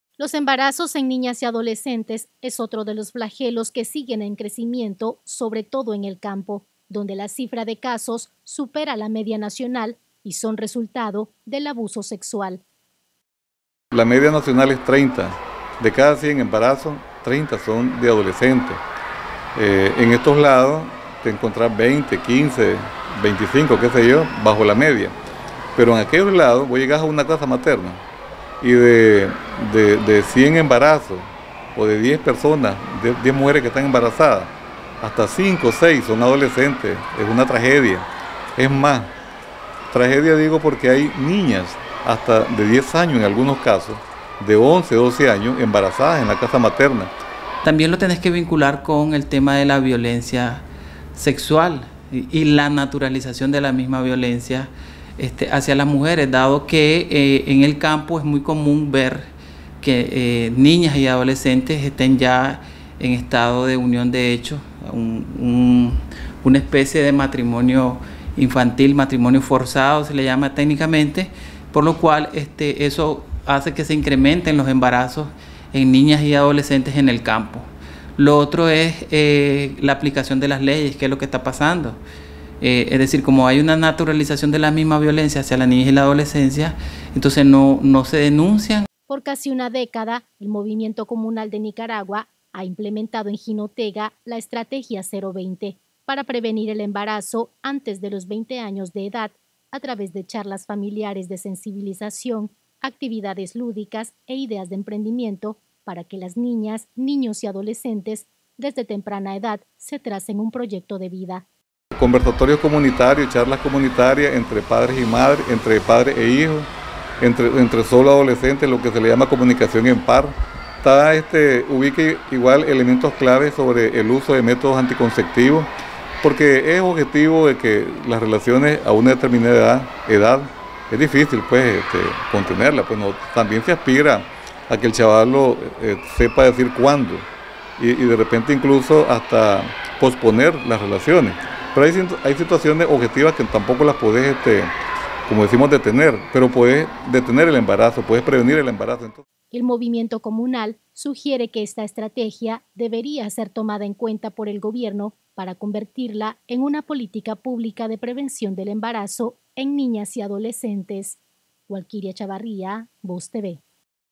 Embarazo: La tragedia de las niñas y adolescentes en Nicaragua
En zonas rurales del país, de cada 10 embarazos 5-6 son de niñas y adolescentes que quedan embarazadas producto de una violencia sexual, según datos del Movimiento Comunal Nicaragüense (MCN).